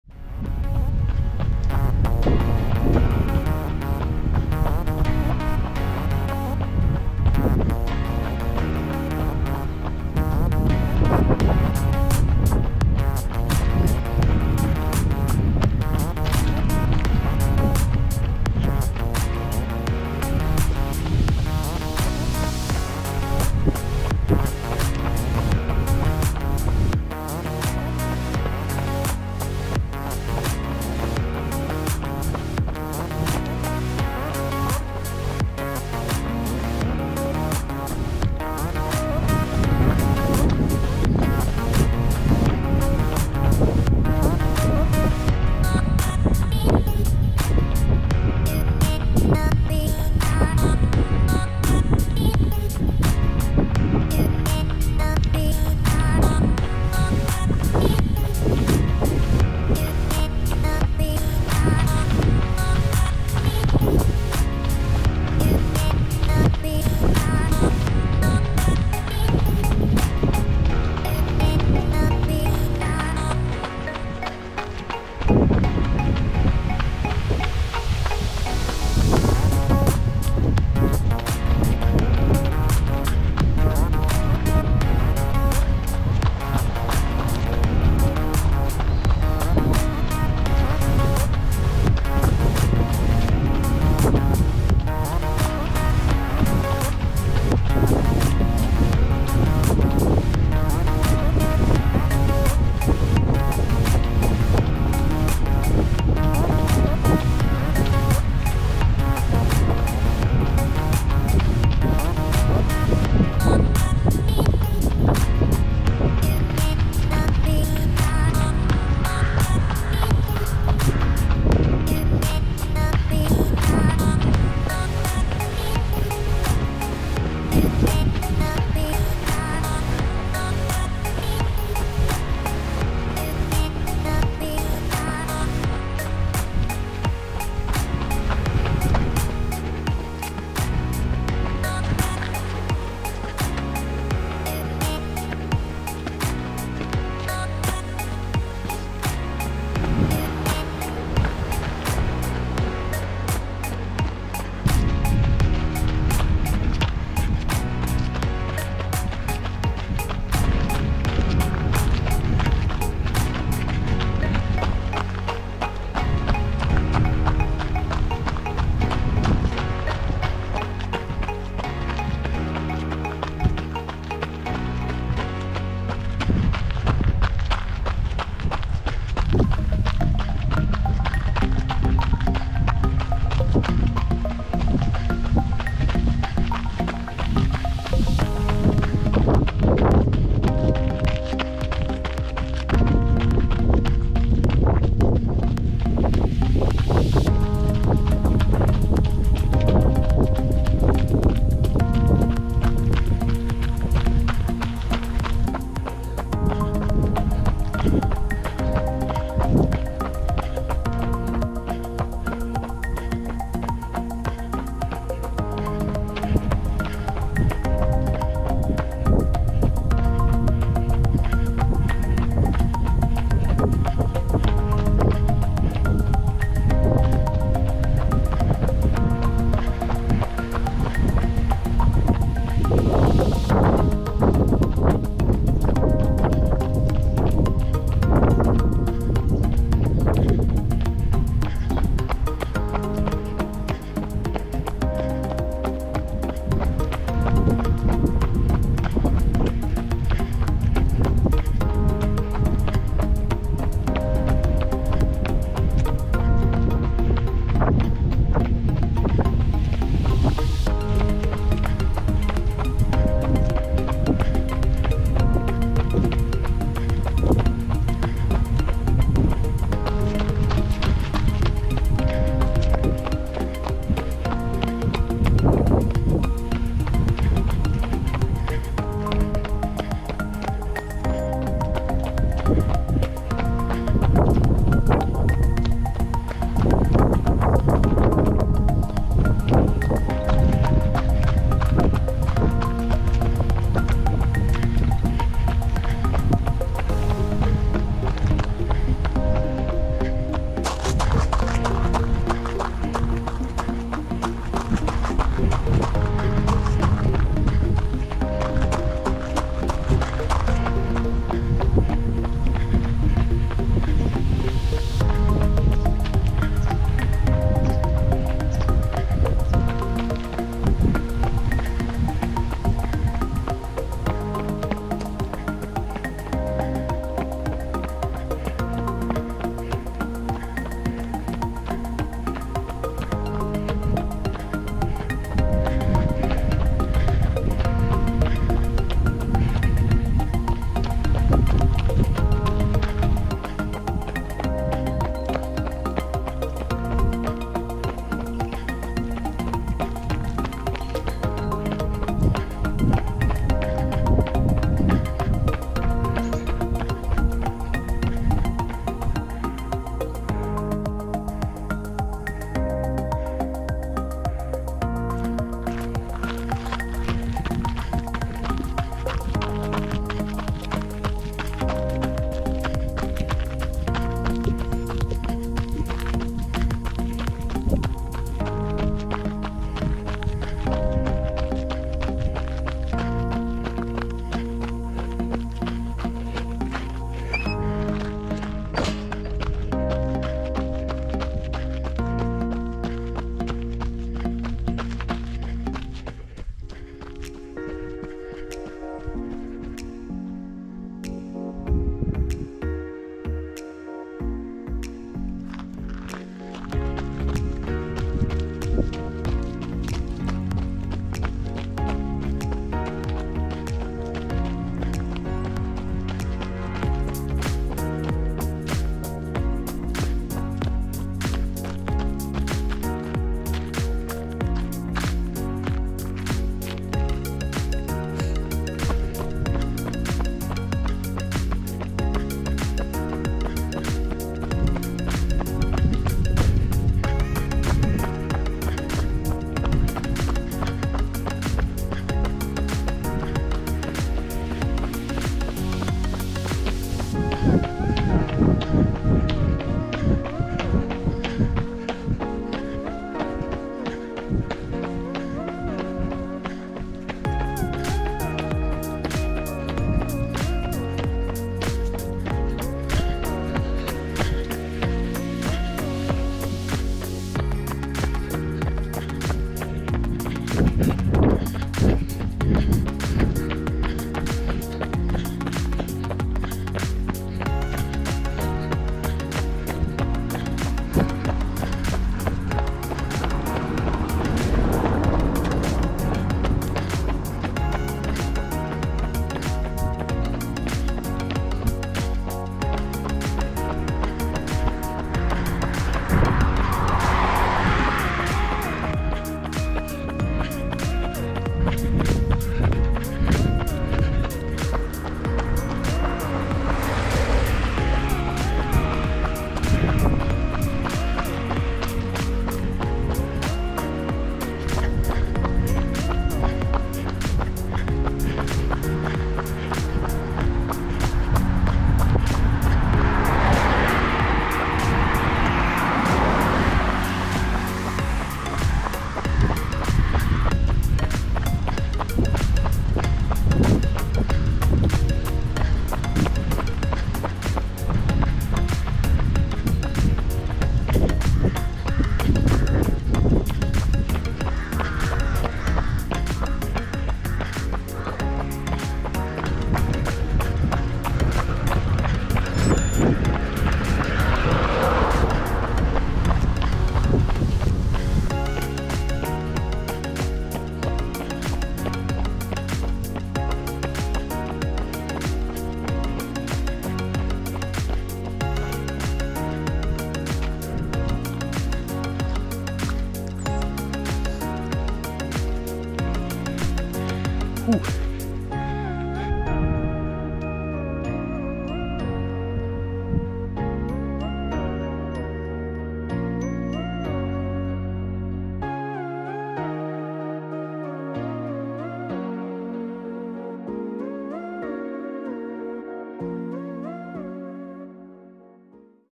Quest For Pace - Run to Explore Bornholm: Tejn and Allinge
A longish run to explore Bornholm. In particular, I run along the coast from Tejn to Allinge, visit a lighthouse, and run back home.